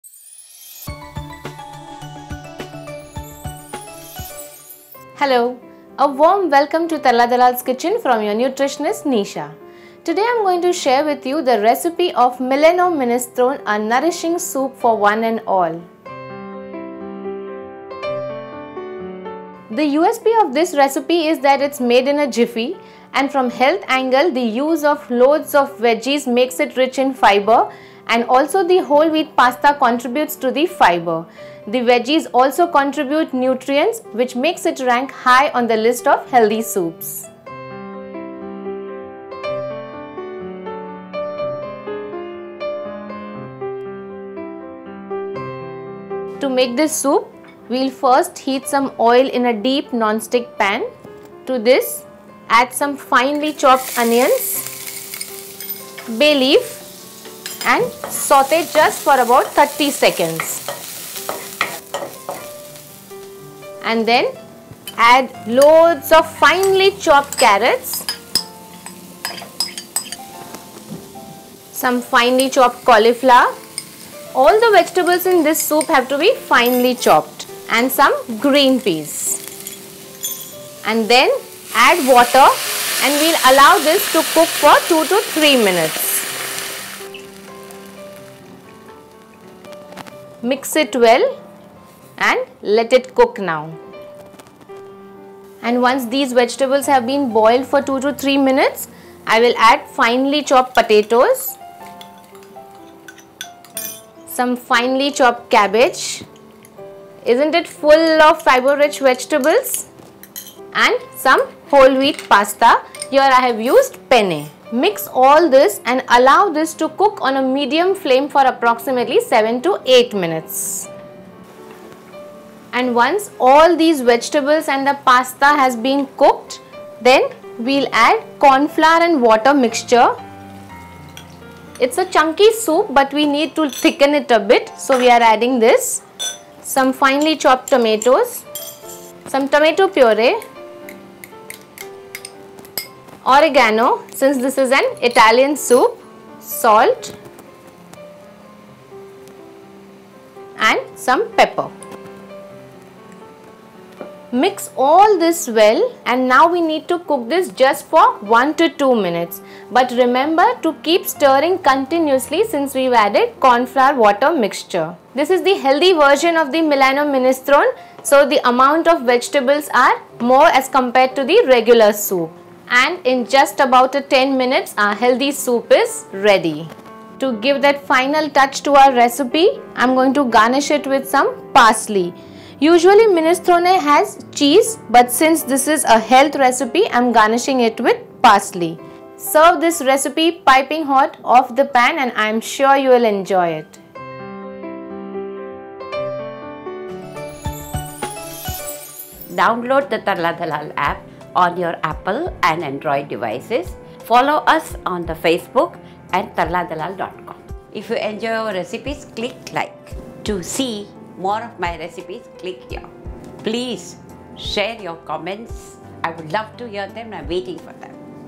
Milano Minestrone (Healthy Soup) by Tarla Dalal
Milano Minestrone, healthy version of minestrone whipped up in a jiffy with loads of fibre rich veggies and perked up with oregano and parsley.

Milano Minestrone

One of the healthy version of minestrone whipped up in a jiffy with loads of fibre rich veggies and perked up with oregano and parsley. Fibre is known to control blood sugar levels and blood cholesterol levels, making this soup a healthy option for one and all.

Serves 6.

¼ cup whole wheat penne
1 tsp dried oregano
1½ tsp oil
2 tbsp finely chopped onions
2 bayleaves (tejpatta)
1½ cups finely chopped carrots
2 tbsp finely chopped cauliflower
2 tbsp green peas
¾ cup peeled and finely chopped potatoes
3 tbsp finely chopped cabbage
1 tbsp cornflour dissolved in ½ cup of water
2 tbsp chopped tomatoes
4 tbsp fresh tomato purée
Salt and freshly ground black pepper (kalimirch) to taste

2 tbsp finely chopped parsley

1. Heat the oil in a deep non-stick pan, add the onions and bayleaves and sauté on a medium flame for 30 seconds.
2. Add the carrots, cauliflower, green peas and 5 cups of water, mix well and cook on a medium flame for 2 to 3 minutes, while stirring occasionally.
3. Add the potatoes, cabbage and whole wheat penne, mix well and cook on a medium flame for 7 to 8 minutes till the vegetables are tender, while stirring occasionally.
4. Add the cornflour-water mixture, tomatoes, tomato purée, oregano, salt and pepper, mix well and cook on a medium flame for 1 to 2 minutes, while stirring continuously.
    Serve hot garnished with parsley.

Nutrient values per serving
Energy: 60 calories
Protein: 1.5 gm
Carbohydrate: 10.3 gm
Fat: 1.4 gm
Fibre: 2.6 mg
Vitamin A: 693.4 mcg